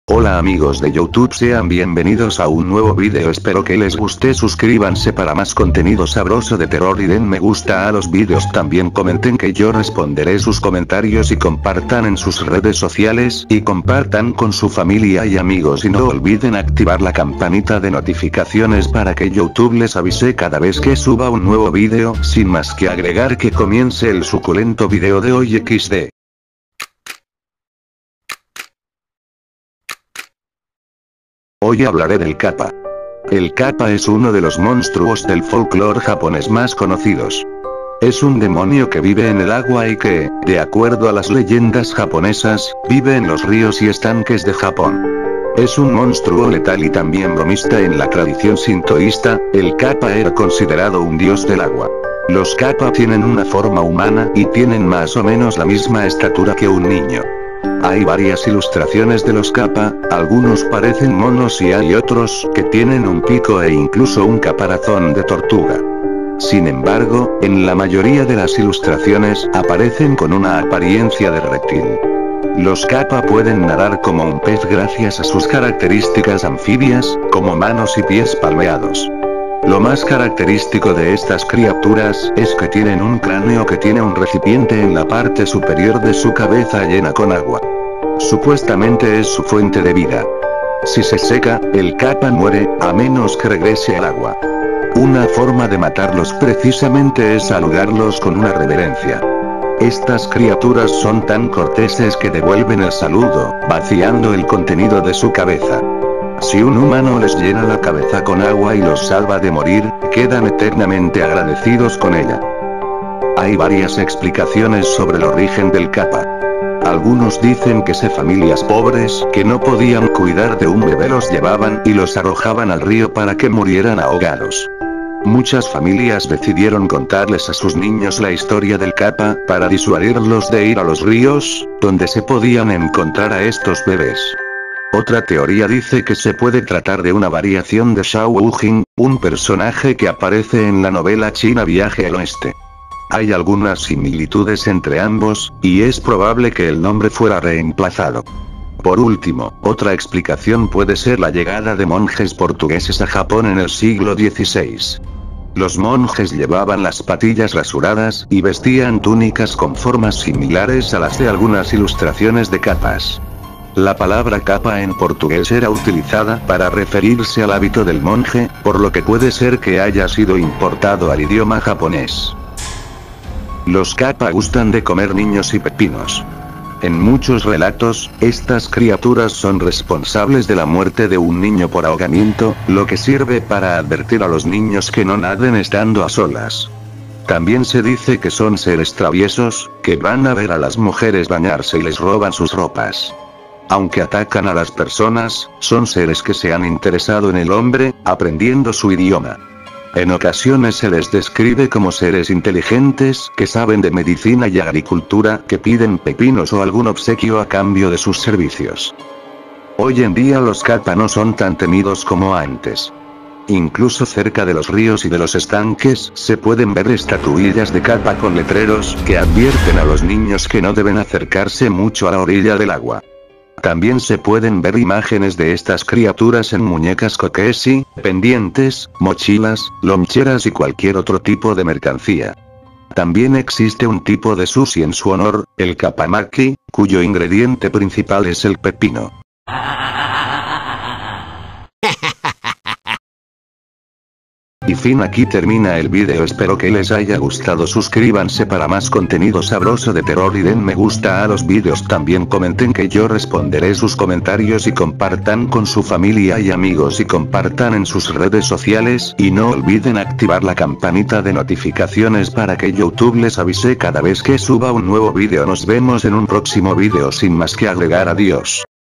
La Leyenda Del Kappa
En Éste Vídeo Les Ablare sobré Él Orígen Del Yokai Kappa TAMBIÉN Les Ablare sobré Los Avitos De Este Yokai Si Quieren Saber Más de Esta Historia Van Este Vídeo